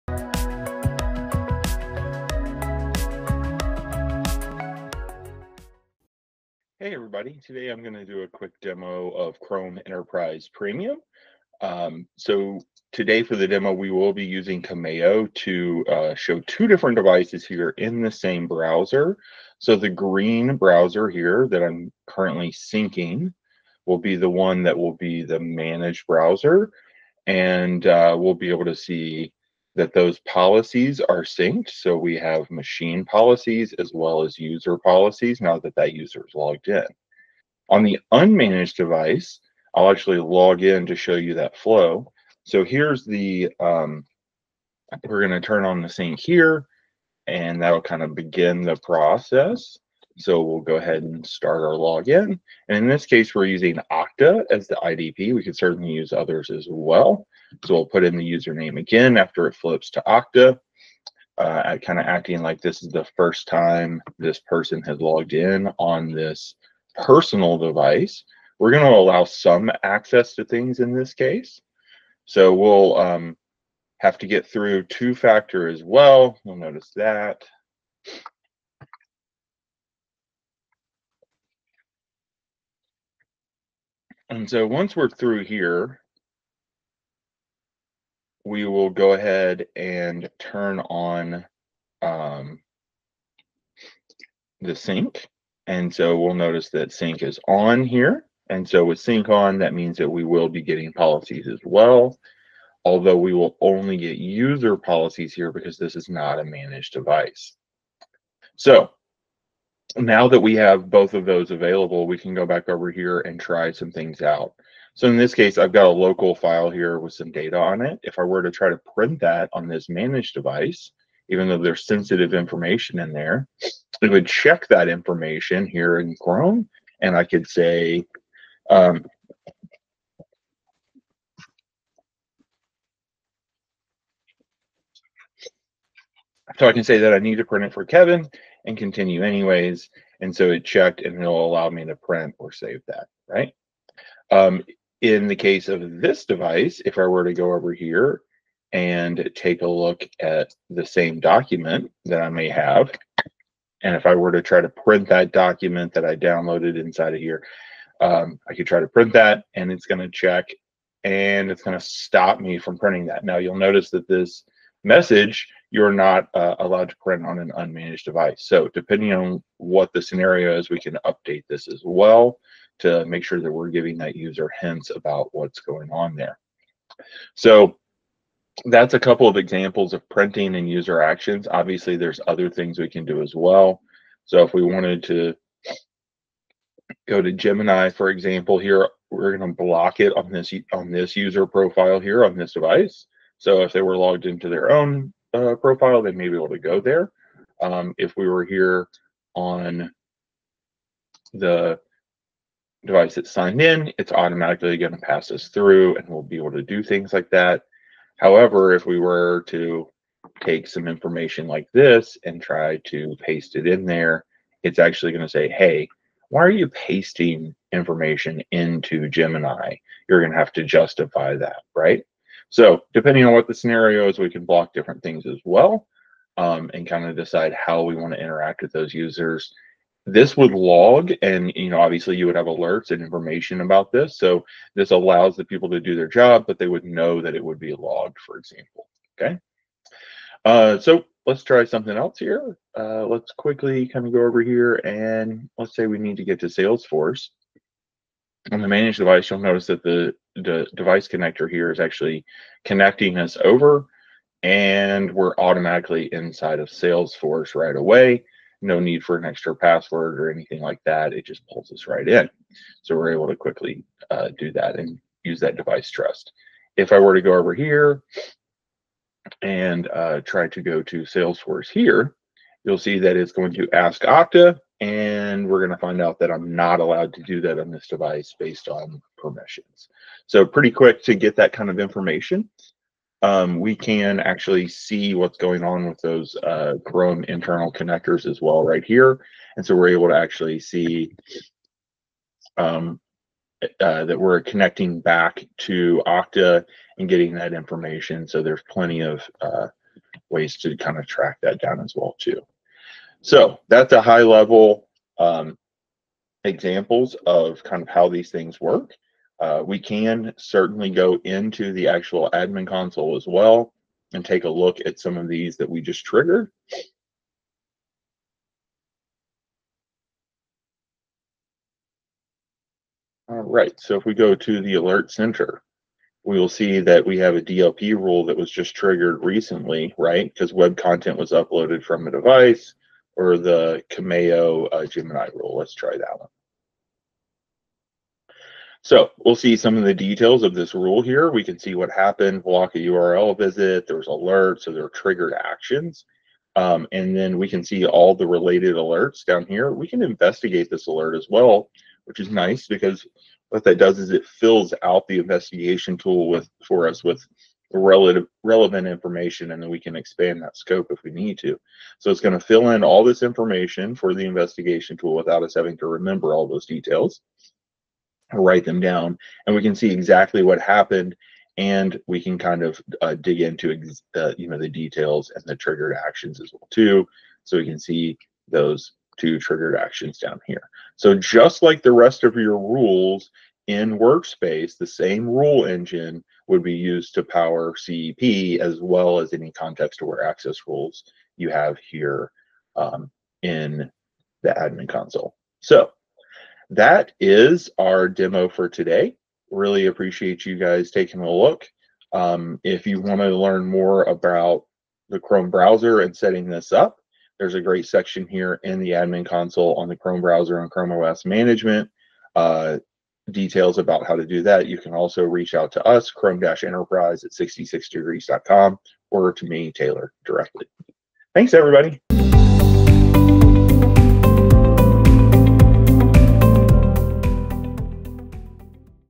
Google Chrome: Enterprise Premium Demo
Led By Taylor Granstaff, Customer Engineer at 66degrees